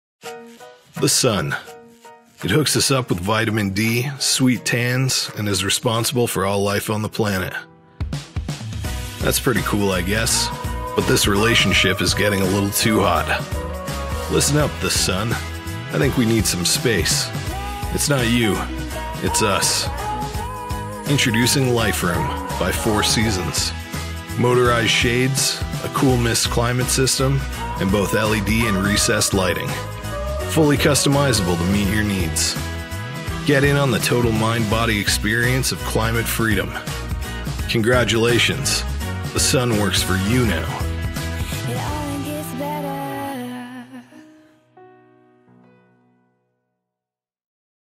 Life Room Four Season Dubai
LIFEROOM BY FOUR SEASON UAE

This is Elegance defined. Four Seasons revives a romantic age of splendor with its Victorian Conservatory. The faceted "bull nose" on this European themed room addition lends it a refined elegance befitting the finest homes but an economical price in keeping with the most humble suburban homes. Transport yourself back to a time of grace and style in the Four Seasons Victorian Conservatory.

Factory No.16, Street 12, Industrial Area, Al Quoz 2 PO. Box 37613 Dubai,UAE